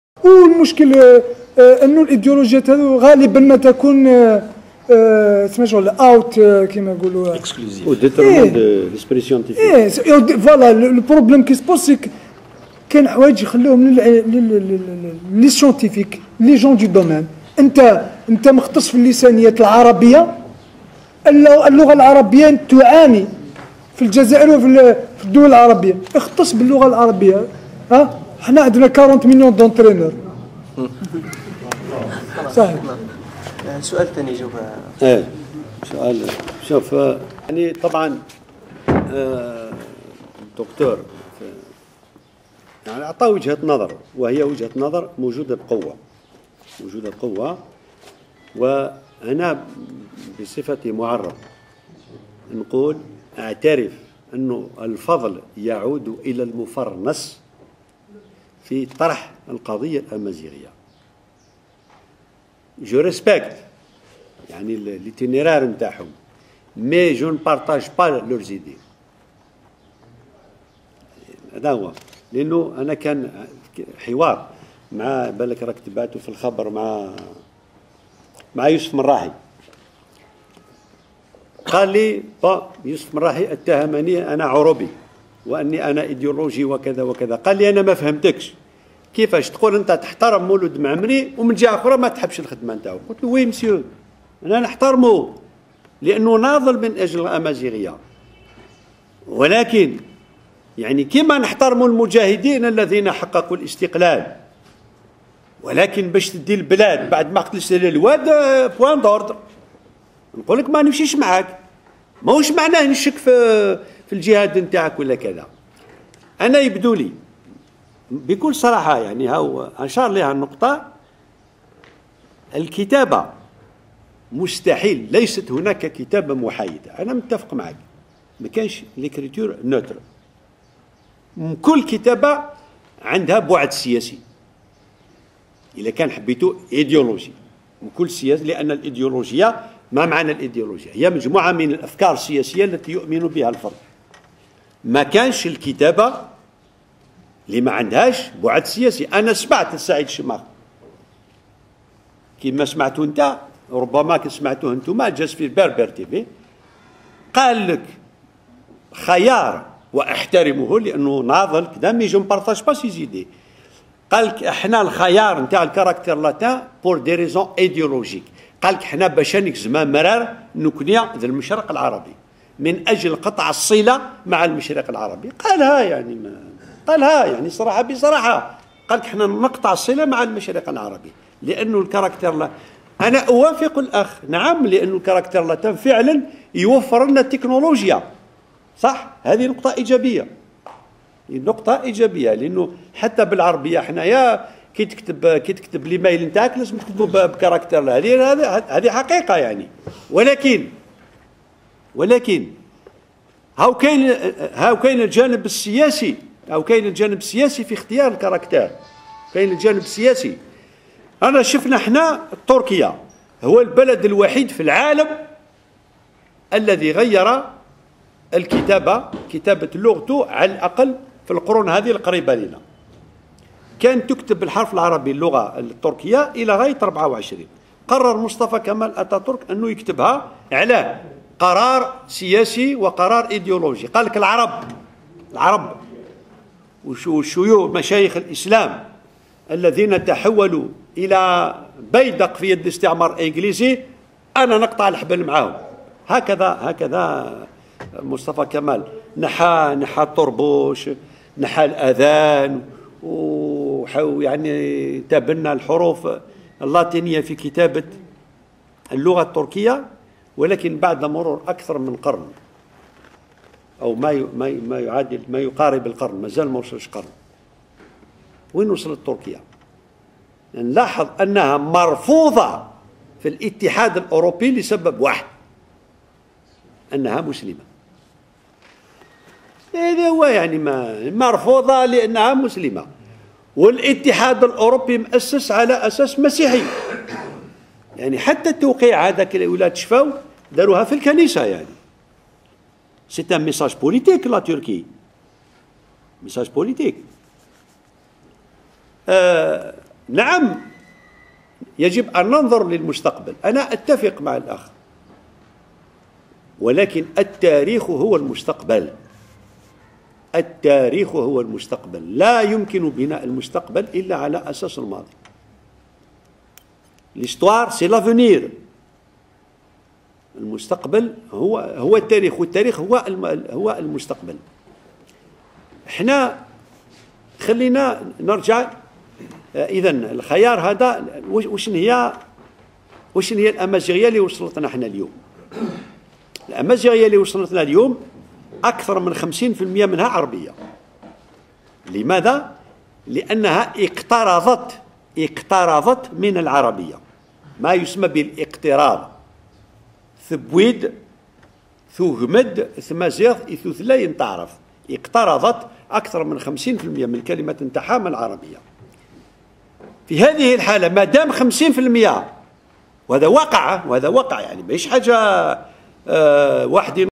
27 04 2013 Débat sur L'identité de la Nation organisé par le conseil scientifique de Jil Jadid 12/22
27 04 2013 à Lakhdaria .Débat aminé par Kouzi Hadj Aissa (membre du Conseil Scientifique de Jil Jadid) présenté par Mr Mohand Arezki Ferad Docteur en histoire et Chbieb Nabil enseignant au département Amazigh a l'université de Bouira